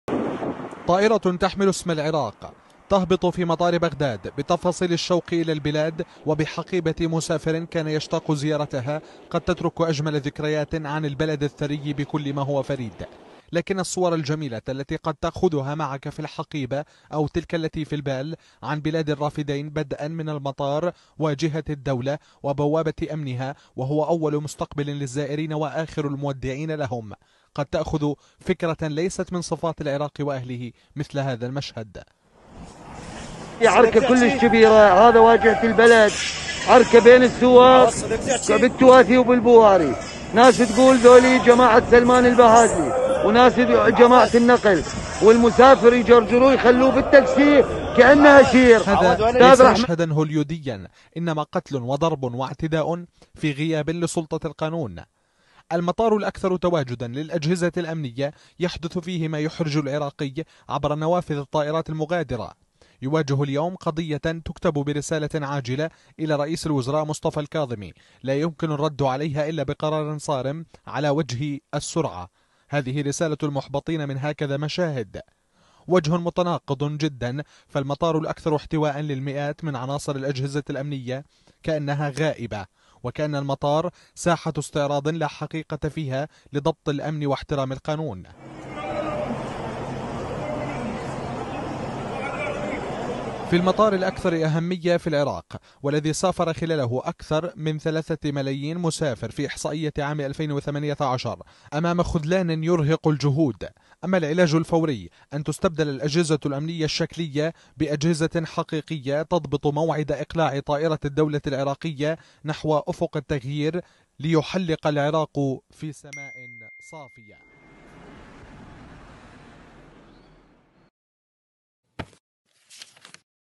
تحذير.. فيديو مفبرك منسوب لقناة التغيير
توضيح من قناة التغيير..
بهدف تضليل الرأي العام.. قامت إحدى الصفحات المغرضة بنشر تقرير لقناة التغيير ووضعت عليه شريط إخباري لا يمت للقناة بصلة..
القناة تحتفظ بحق الرد قانونيا على كل من يستخدم اسمها لأغراض تسيء للعمل الصحفي والمهنية الإعلامية.. وتدعو متابعيها الى توخي الدقة في نقل كل ما يتعلق بالقناة من أخبار او مقاطع مصورة تنشر على منصاتها الإعلامية...